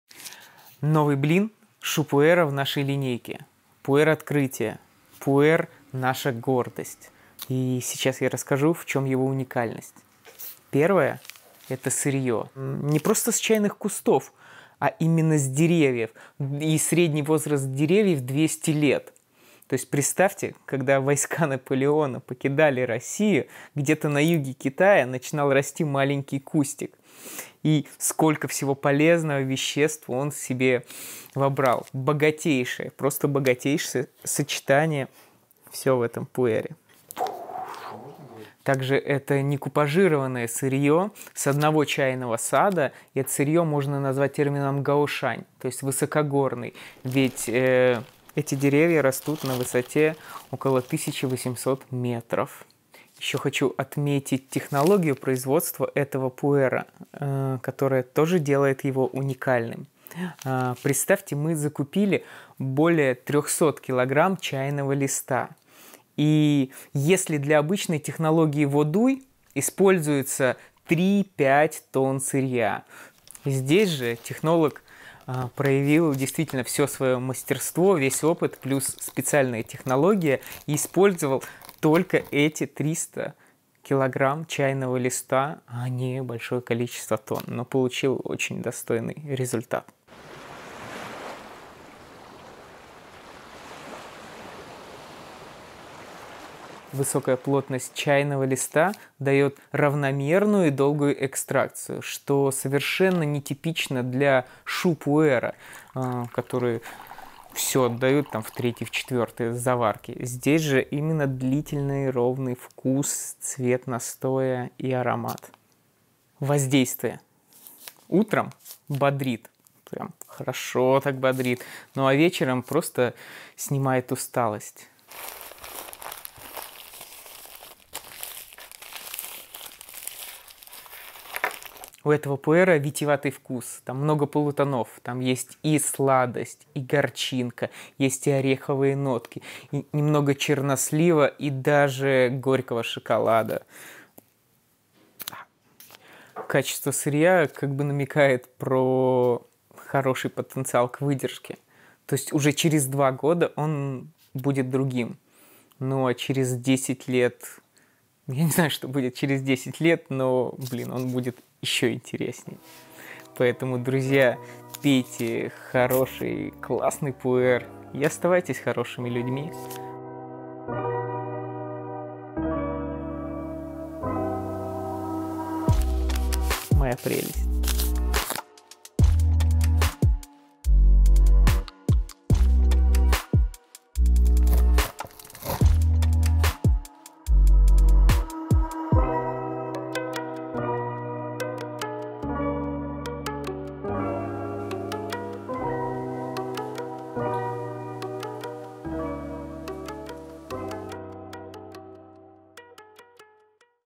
Шу Пуэр «The Art Of Tea» - Хэй Лун «Черный Дракон» | Китайский чай
Обзор фирменного шу пуэра от Art of Tea: блинчик 200г, сырьё со старых чайных деревьев, высота произрастания 1800м. Андрей Власов, бессменный руководитель нашего чайного проекта, расскажет подробно, как подбирали сырьё, как делали дизайн обложки, в чём особенности изготовления этого пуэра.

Как «Чёрный дракон» попал в бамбук? Читайте в нашем чайном блоге